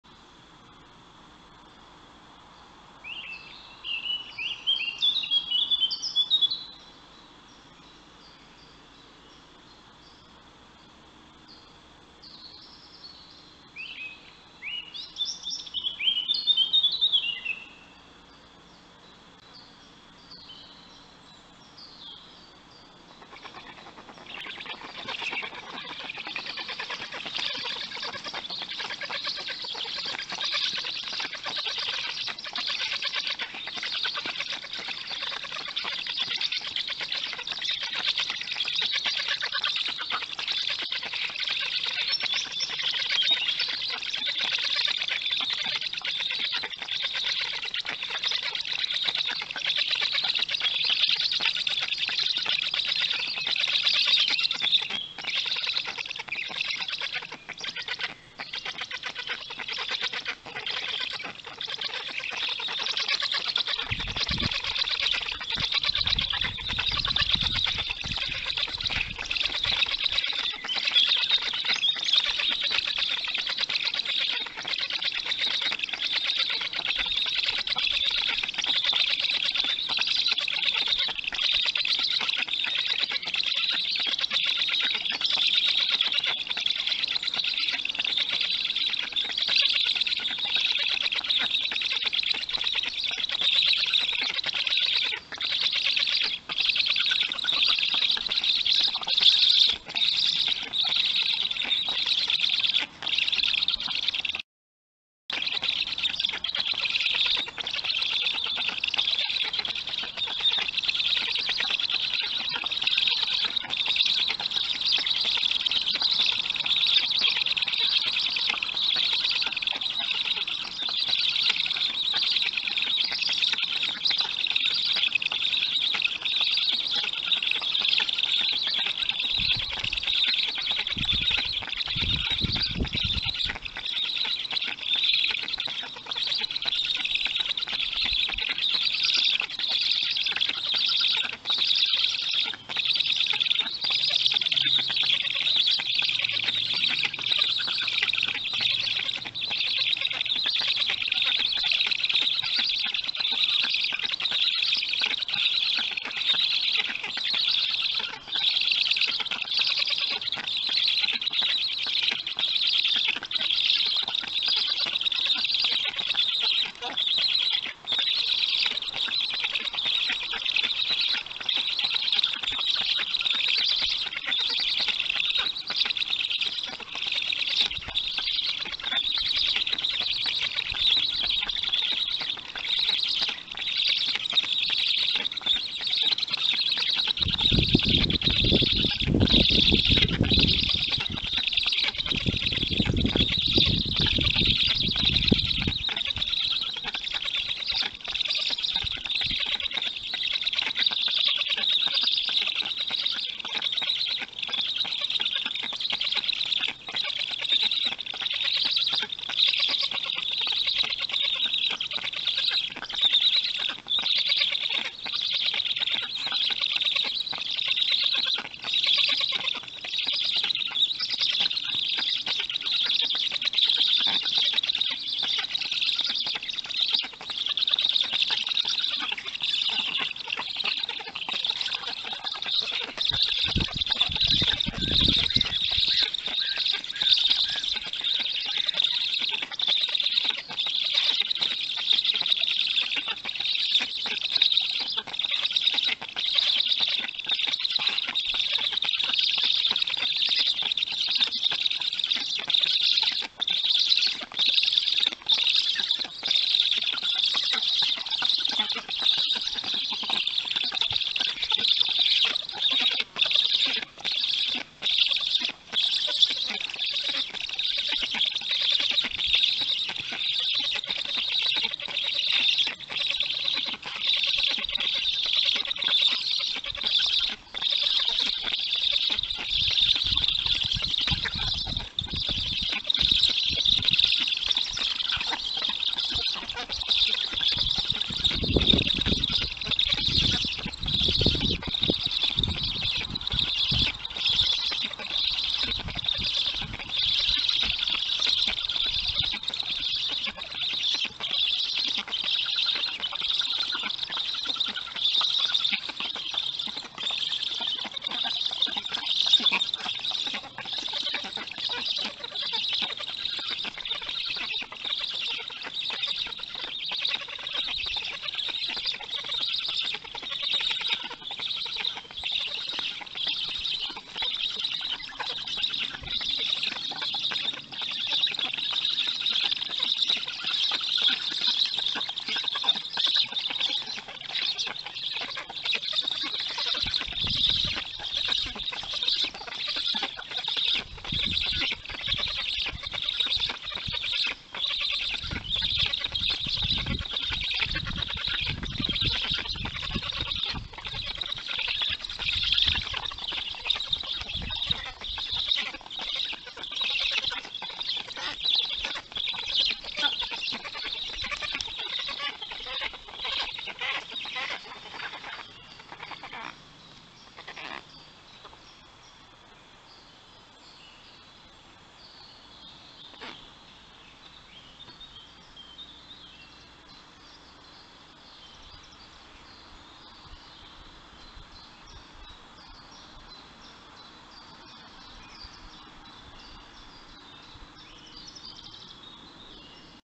Black Stork Cam in Poland | Lunch is served | 06-27-20 | 2:00 p.m.
At 2:00 p.m. on 06-27-20, one of the adult storks returns to feed its three large storklets.  After belaboring the ability to regurgitate what appear to be small minnows, the adult stork delivers the food onto the nest in about 3 efforts.  With no more duty at the nest, the adult flies away to search for more food to offer later in the day.